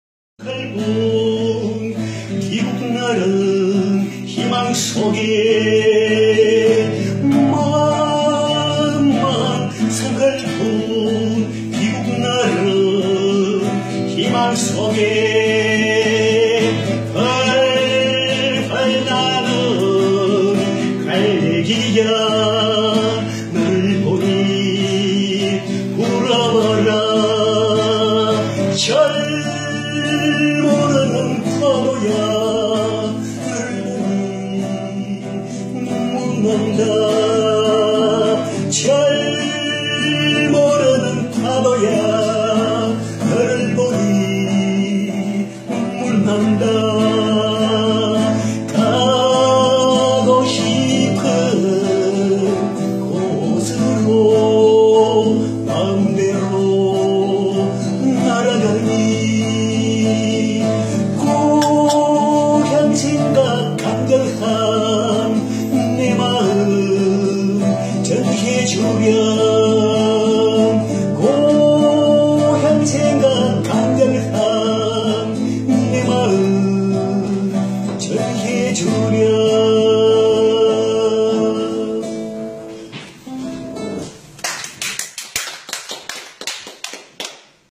Авторская песня Романа Хэ
Роман Хе исполняет авторскую песню. Здесь всего лишь одно видео, фрагмент его выступления перед гостями. Меня он поразил синтезом корейской поэзии с русской, европейской, мировой, как хотите, традицией.